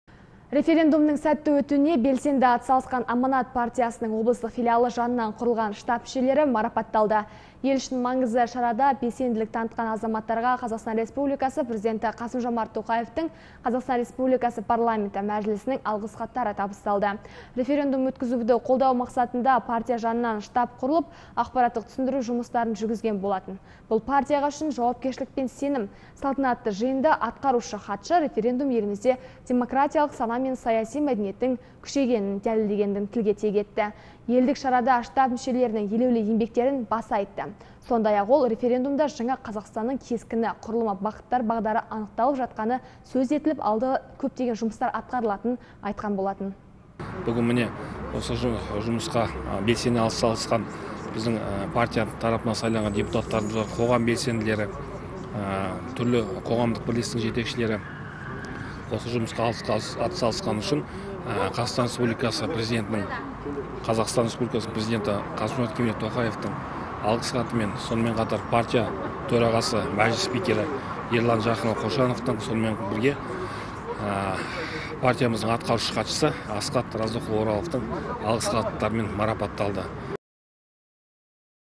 Белсенді азаматтар марапатталды
Референдумның сәтті өтуіне белсенді атсалысқан «Аманат» партиясының облыстық филиалы жанынан құрылған штаб мүшелері марапатталды. Ел үшін маңызды шарада белсенділік танытқан азаматтарға ҚР президенті Қасым-Жомарт Тоқаевтың, ҚР парламенті мәжілісінің алғыс хаттары табысталды.